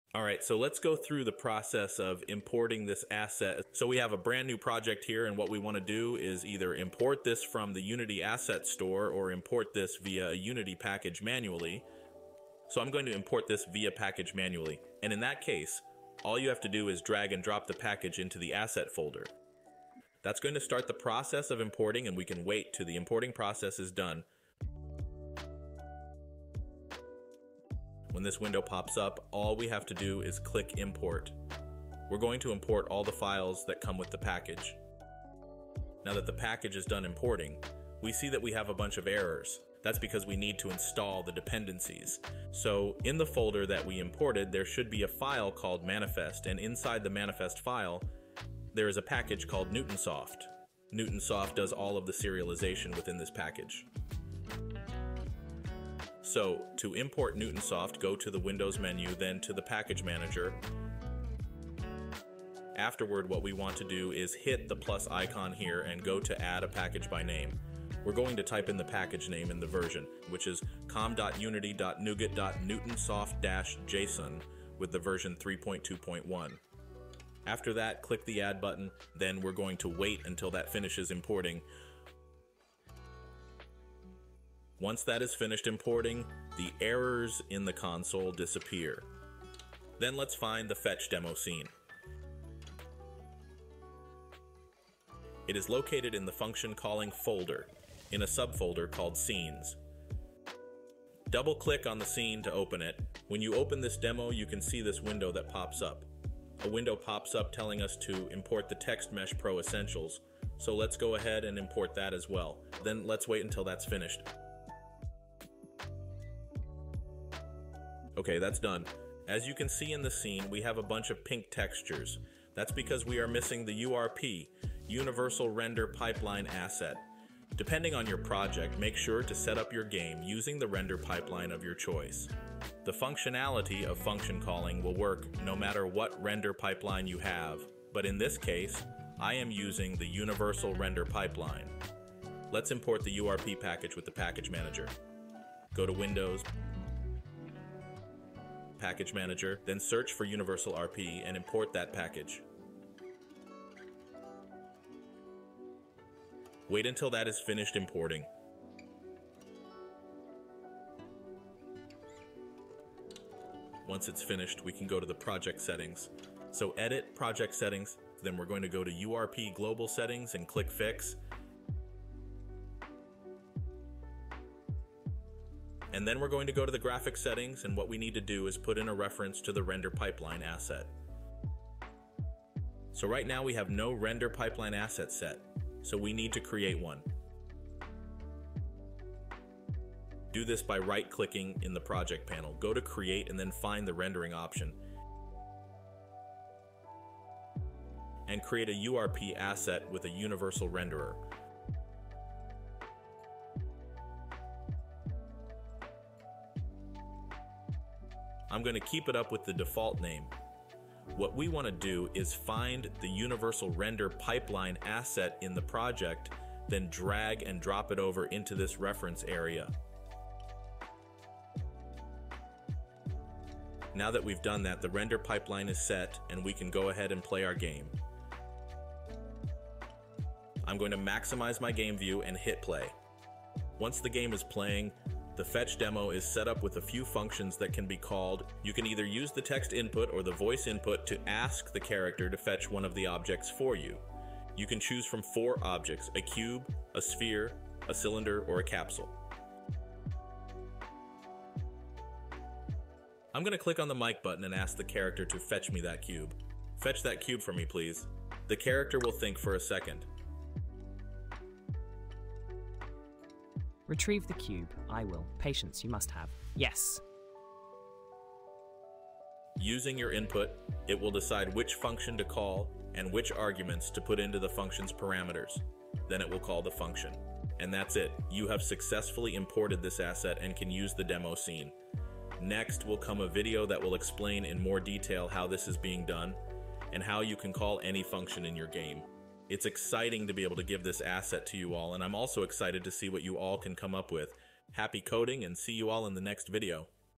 Function Calling For Unity
Unlock the full potential of your Unity game with the OpenAI API! Learn how to leverage its capabilities to make informed decisions about which functions to call within your game environment. By providing contextual information about each function within Unity, you enable the AI model to suggest the most suitable function along with its parameters. Whether you're inputting text or voice commands, the model can swiftly respond with the recommended function and the necessary arguments for each parameter. Elevate your game development experience today!